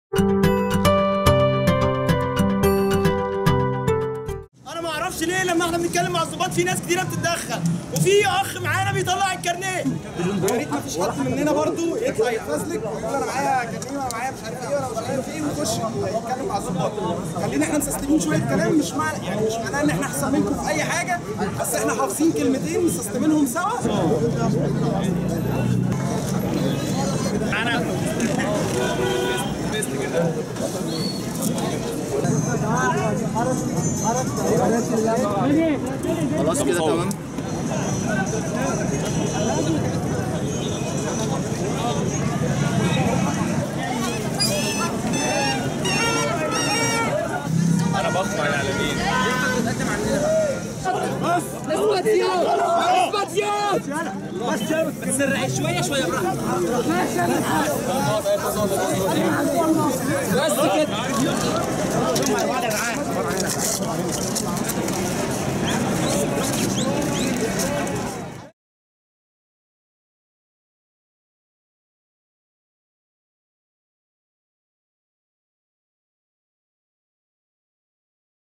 «التحرش بالمتحرشين» تواصل حملتها ثالث ايام العيد وسط مناوشات مع الشرطه
تقرير :عمر ثابت
مونتاج :يوسف ناصر